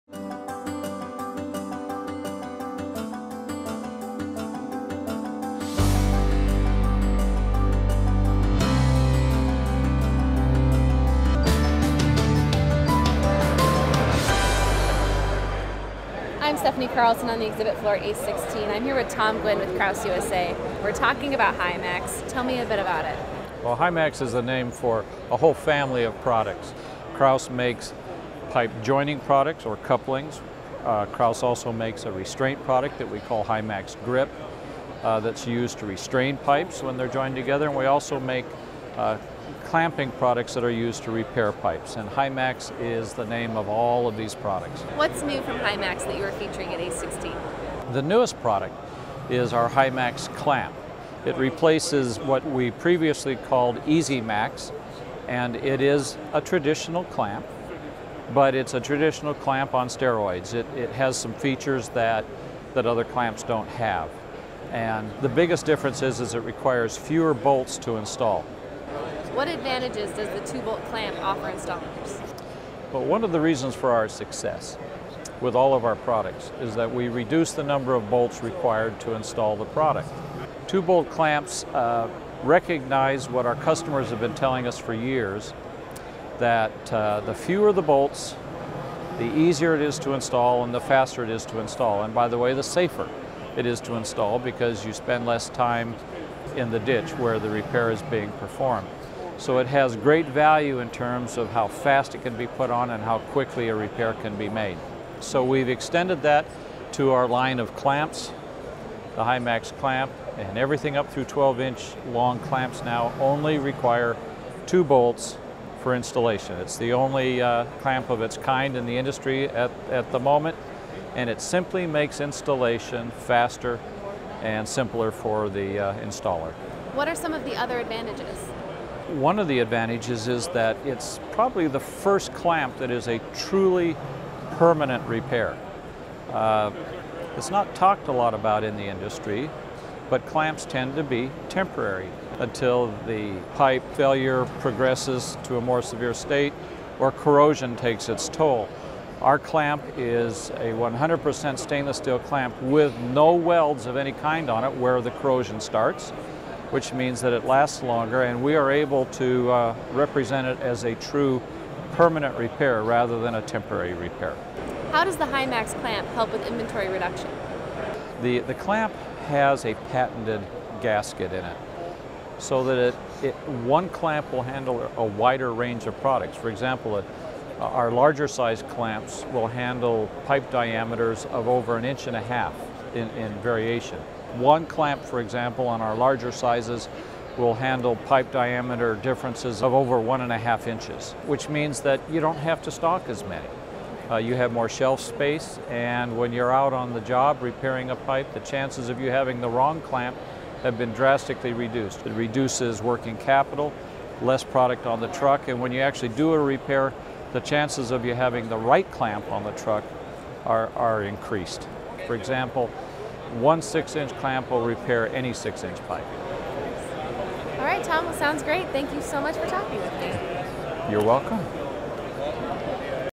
Krausz USA at ACE16
An overview of HYMAX products at ACE16 including the HYMAX Clamp, a new product that can quickly and easily repair holes and longitudinal cracks in water and wastewater pipes.